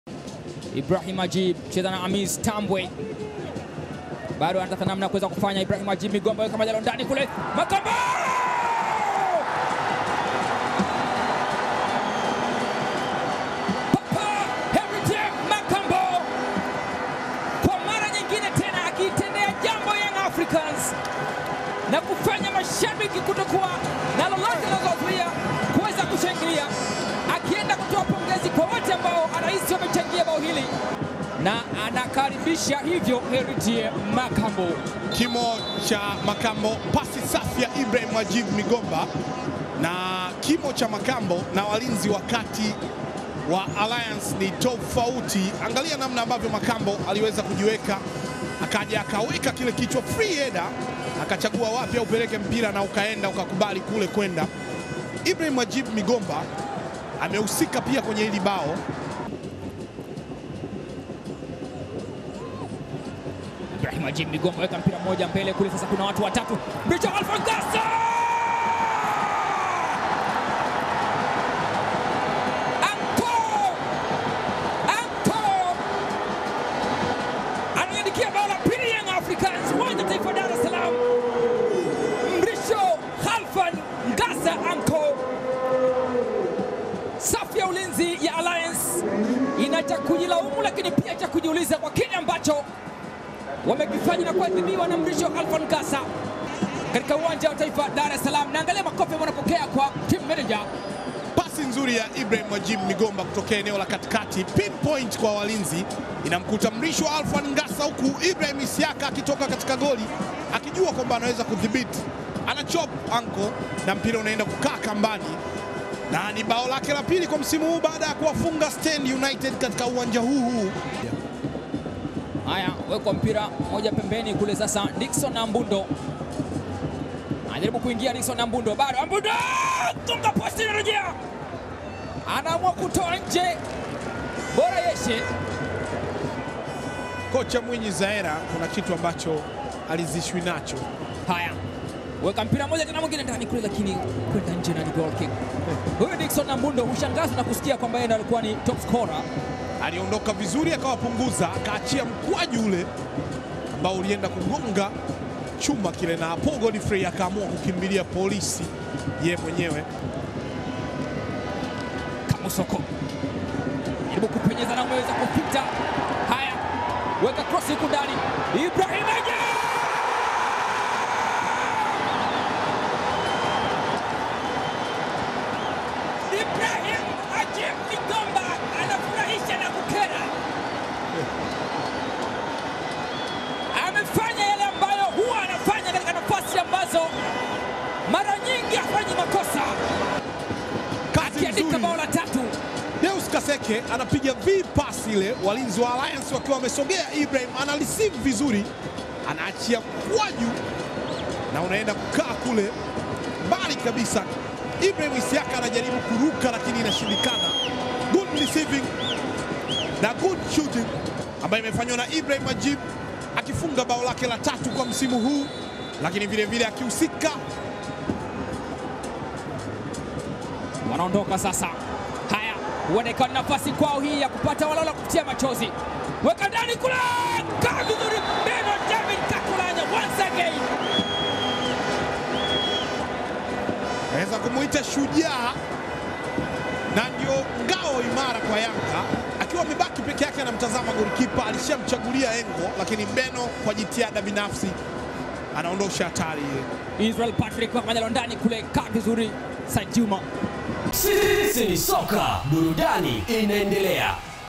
Magoli yote: Yanga ilivyoipa Alliance kipigo cha mabao 3-0 (TPL – 20/10/2018)
Mabingwa wa kihistoria wa Tanzania Bara Yanga SC leo wameiduwaza Alliance kwa kuwa kipigo cha mabao 3-0 katika mchezo uliopigwa kwenye Uwanja wa Taifa Dar es Salaam.

Yanga licha ya kuzidiwa umiliki wa mpira, walianza kuishushia kichapo Alliance dakika ya 18 ya mchezo kupitia kwa Heritiel Makambo akimalizia kwa kichwa krosi ya Ibrahim Ajibu.

Ajibu pia alihusika kwenye bao la pili kwa kutoa pasi ya mwisho kwa Mrisho Ngasa aliyefunga bao la pili dakika ya 25.

Deus Kaseke aliyeingia kipindi cha pili kuchukua nafasi ya Mrisho Ngasa, alitoa pasi ya mwisho kwa Ibrahim Ajibu dakika ya 86, ambaye alifunga bao la tatu na kukamilisha ushindi huo mnono wa Yanga ukiwa ni ushindi wa sita kwenye uwanja wake wa nyumbani.